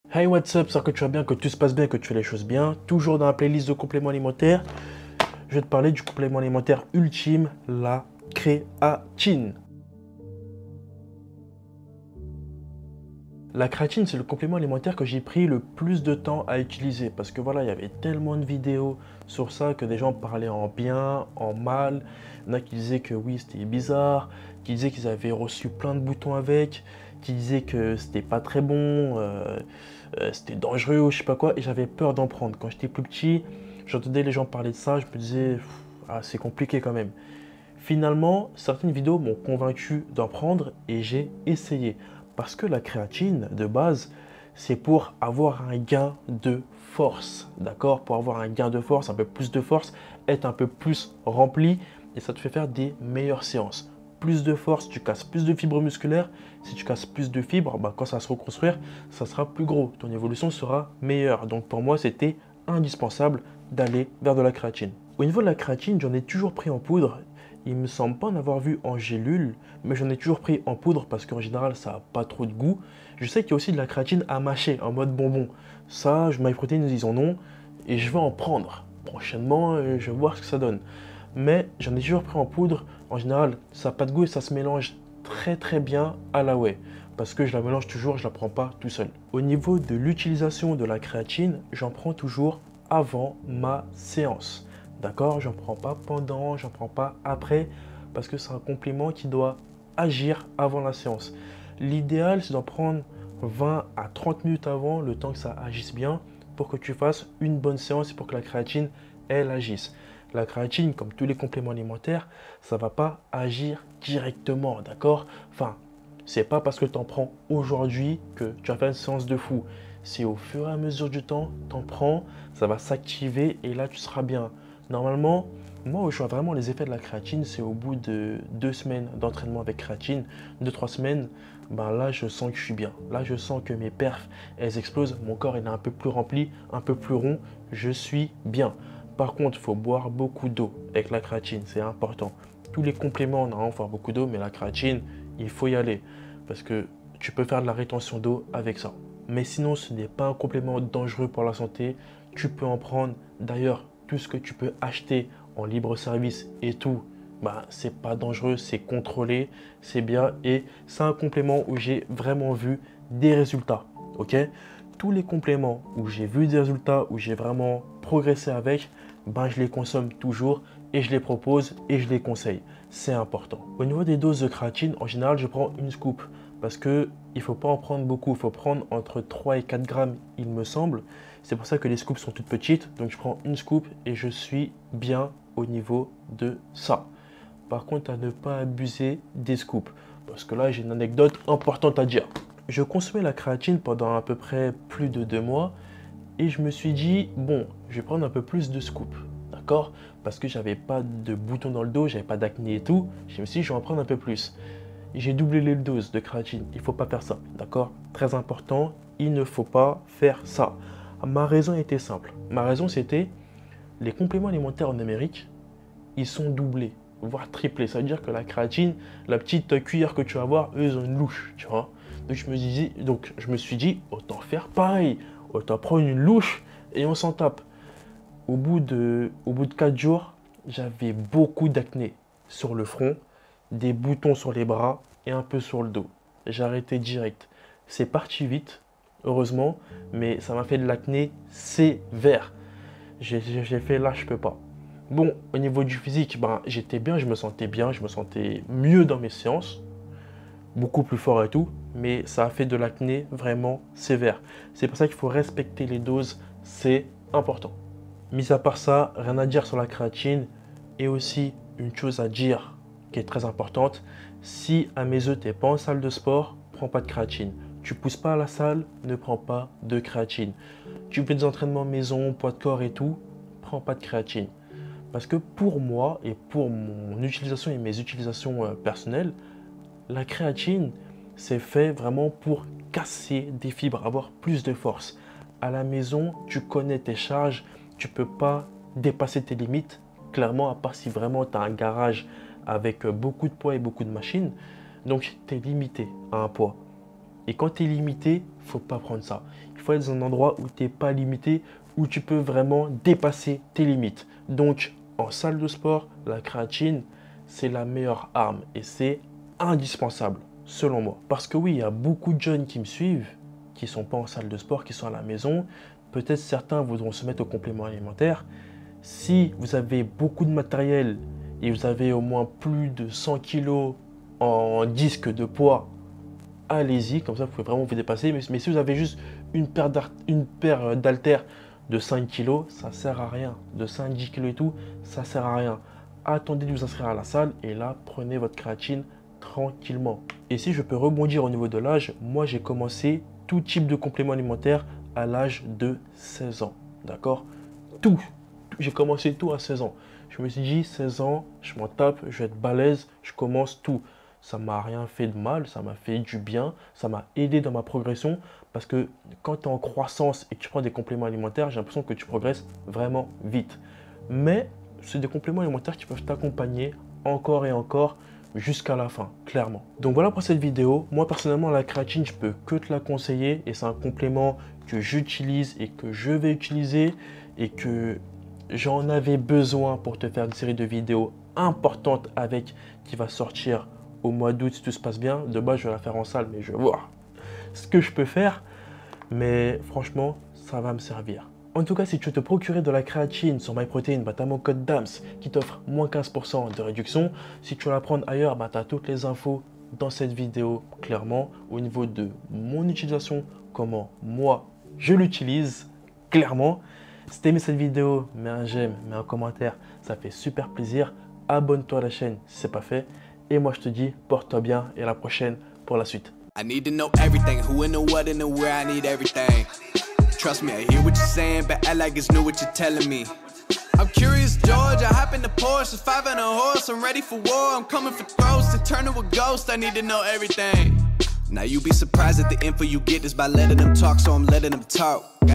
COMMENT JE PRENDS LA CREATINE !?
Aujourdhui petite videos pour te montrer comment je prend la creatine dans mes seances !